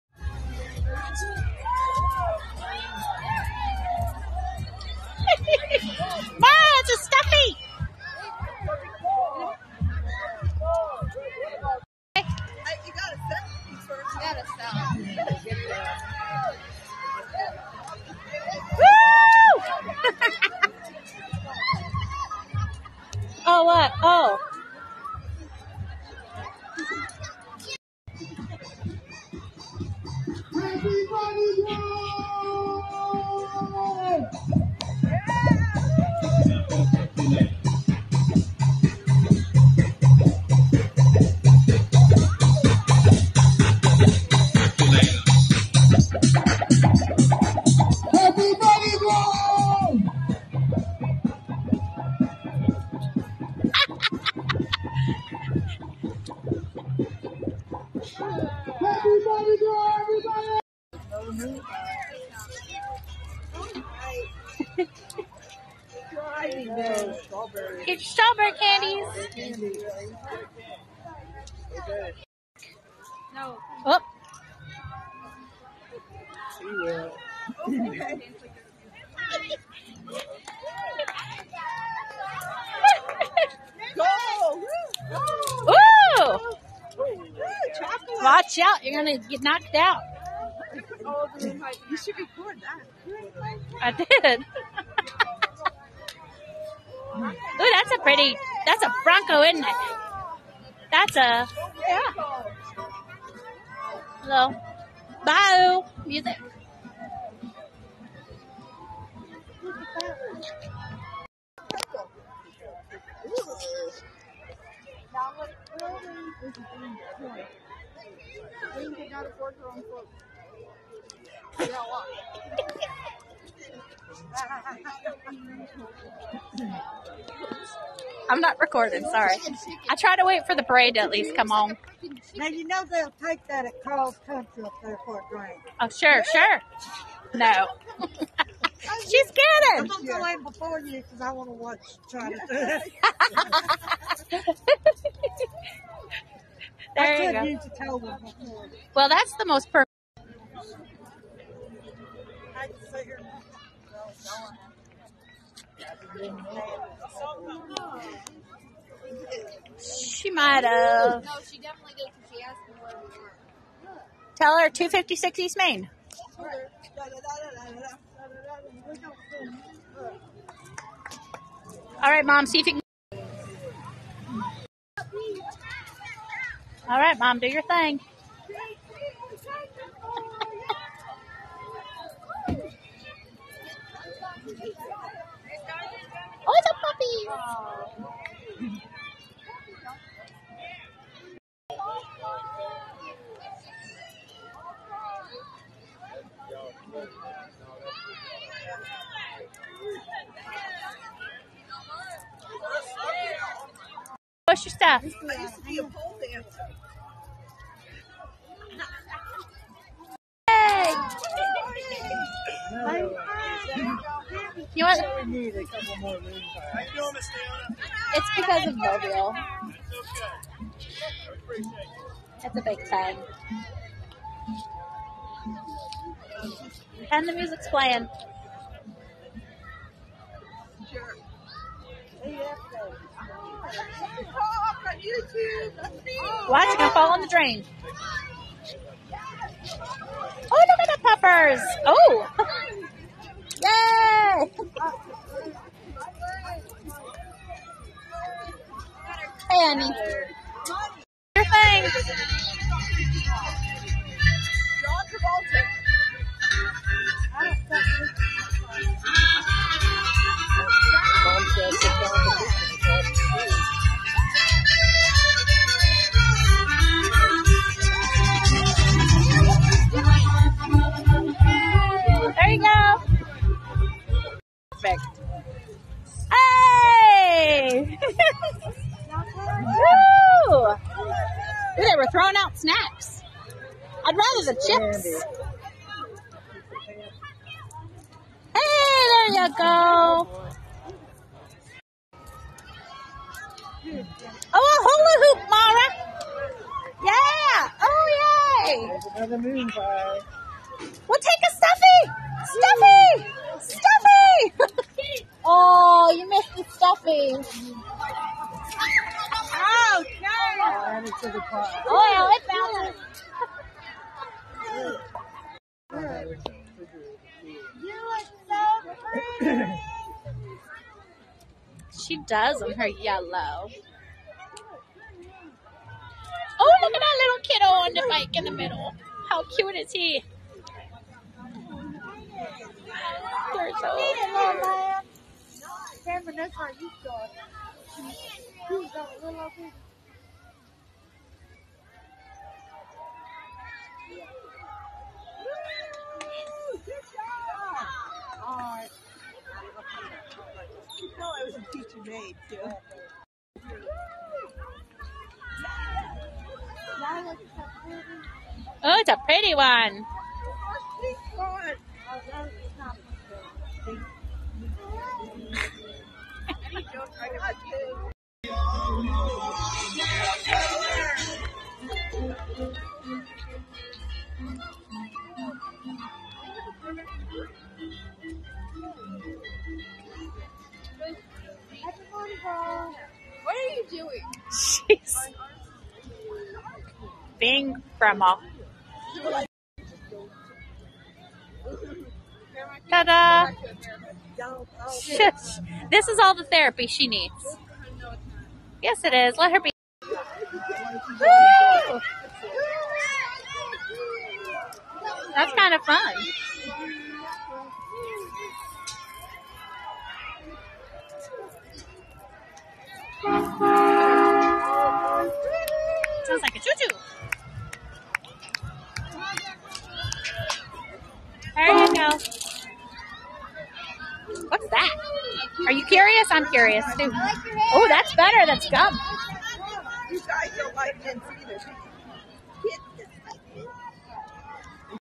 Fat Tuesday, Happy Mardi Gras Parade
I had a great time at the Prattville, AL Mardi Gras Parade. Just keep dancing!